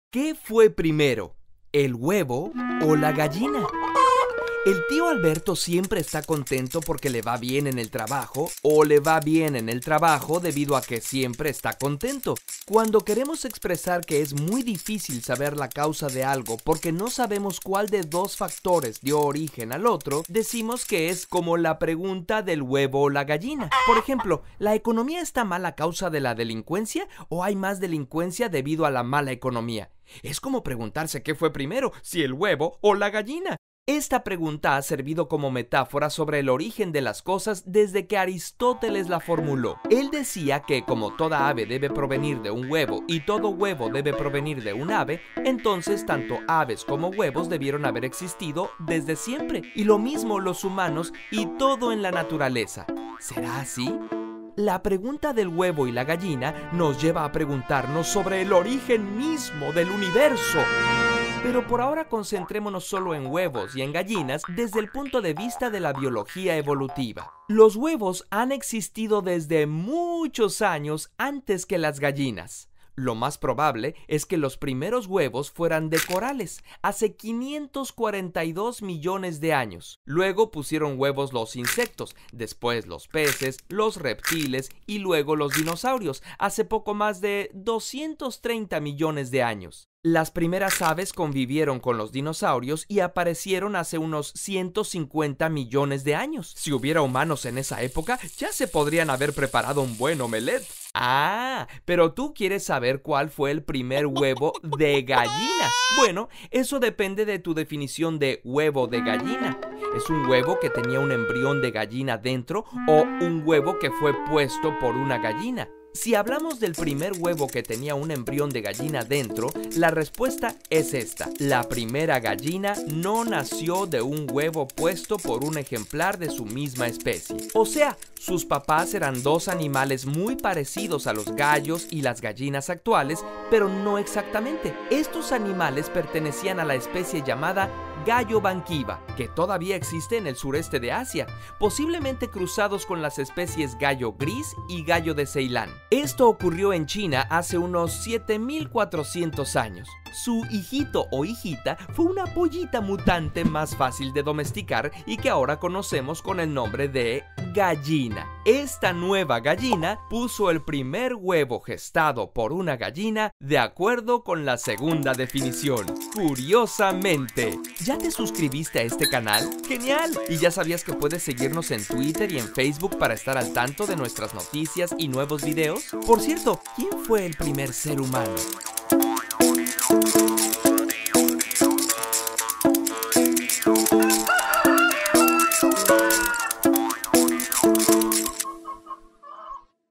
¿Qué fue primero, el huevo o la gallina?
¡El gran misterio ha sido descubierto! ¡Tenemos la respuesta a la pregunta que nunca ha sido contestada! ¿Si la gallina fue primero, de dónde salió? ¿Si el huevo fue primero, quién lo puso?

La pregunta "¿Qué fue primero, el huevo o la gallina?" ha servido como metáfora sobre el origen de las cosas desde que Aristóteles la formuló. Él decía que, como toda ave debe provenir de un huevo y todo huevo debe provenir de un ave, entonces tanto aves como huevos debieron haber existido desde siempre, y lo mismo los humanos y todo en la naturaleza. ¿Será así? La pregunta del huevo y la gallina nos lleva a preguntarnos sobre el origen mismo del universo.

Dirección y guión: Tonatiuh Moreno
Ilustraciones: Ruy Fernando Estrada
Voz: Javier Lacroix

Estudio de genética evolutiva sobre las aves y los demás vertebrados